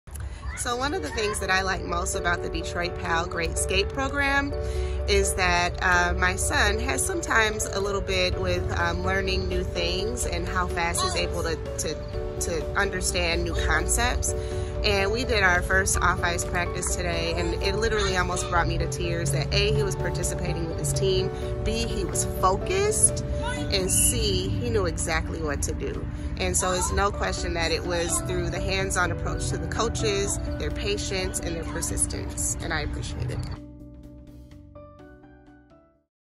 Coachable Moments
There were and continues to be many inspiring Coachable Moments in 2022. Through team play, PAL coaches guided, taught, and inspired the youth of Detroit.

Plan to give our youth the gift of a GREAT coach & invest in the future of Detroit on Giving Tuesday, Nov. 29th